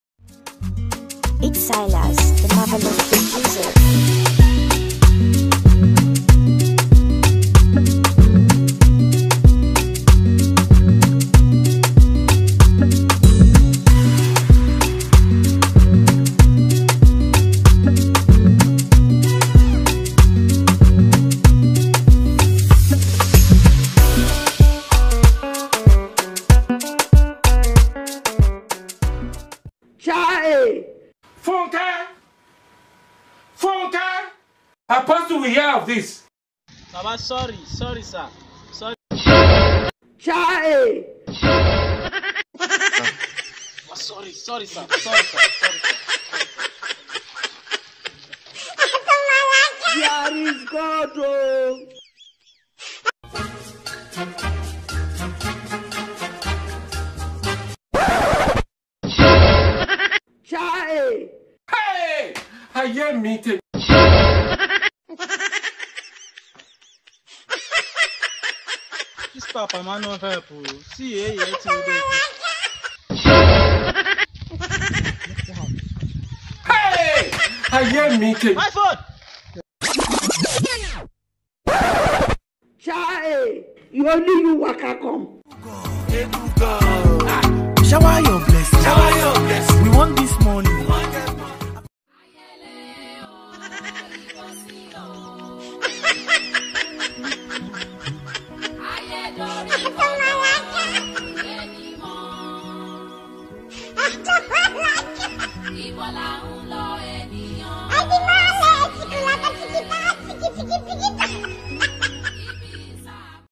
BAD_MARKET (XPLIOT COMEDY)(Xplit Comedy)
A short funny hilarious videos dat talks abt de bad day for unfaithful Hacker...